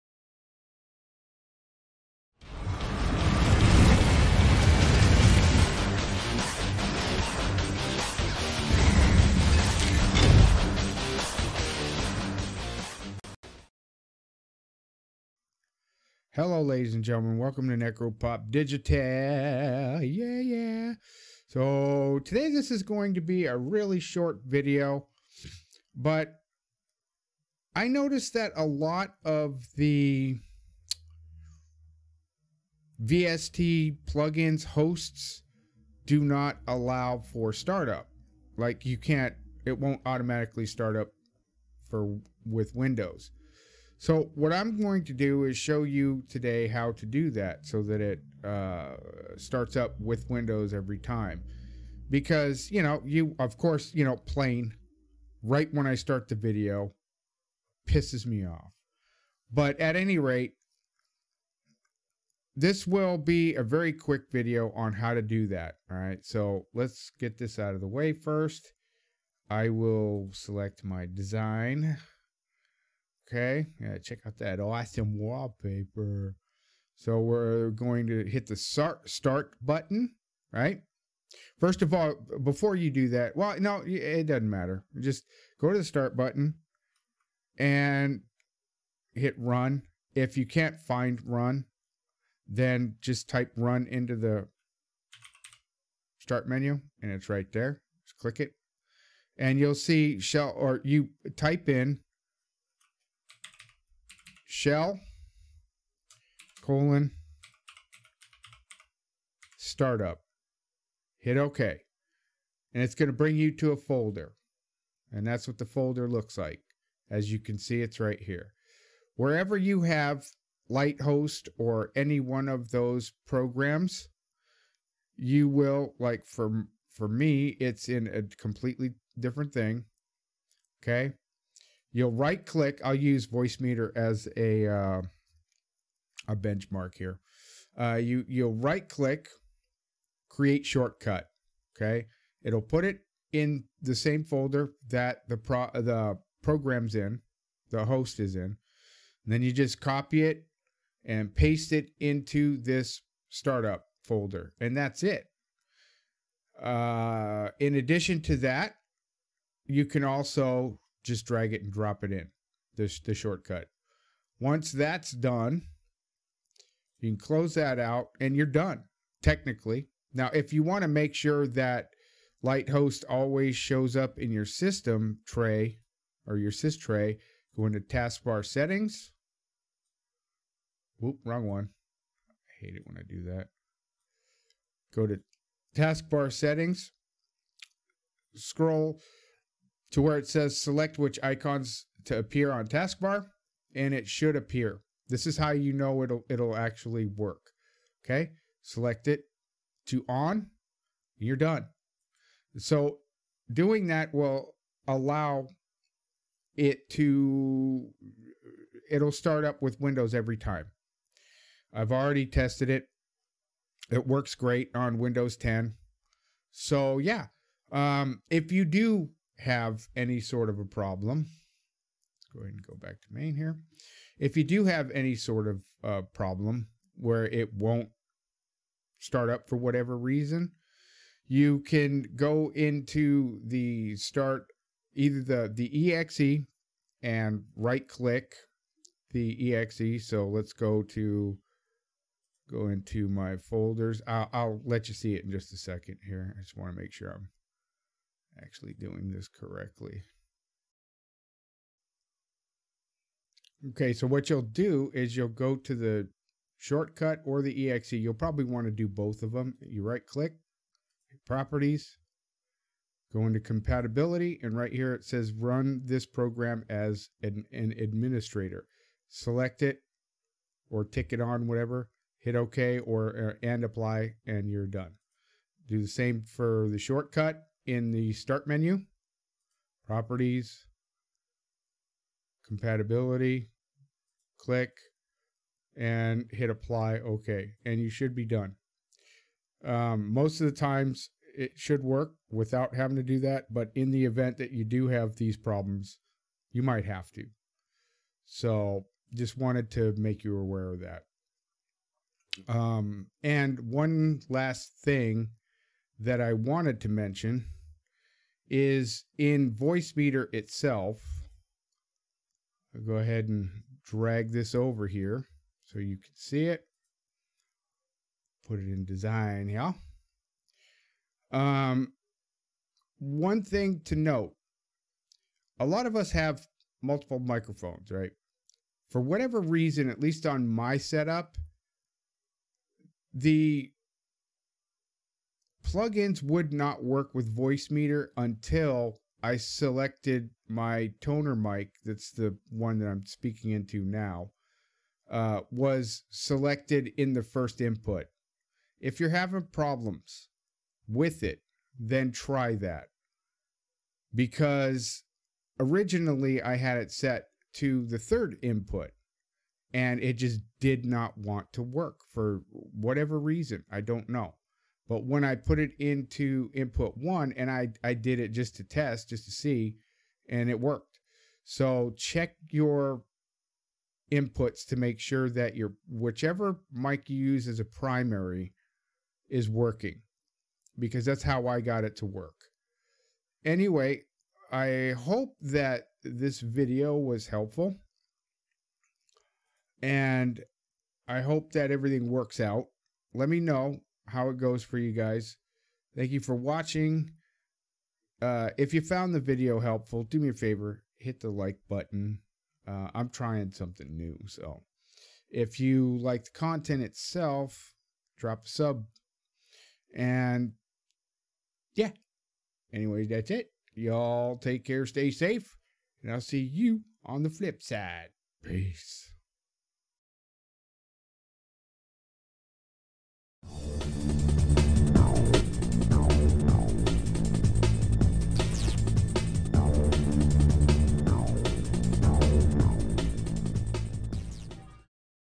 How To Get VST Plugin Hosts To Start With Windows 10
This video will provide information on how to add VST plugin hosts to Windows startup even if they don't have the feature. I've tested this with Light Host, but this method should also work for any other VST host.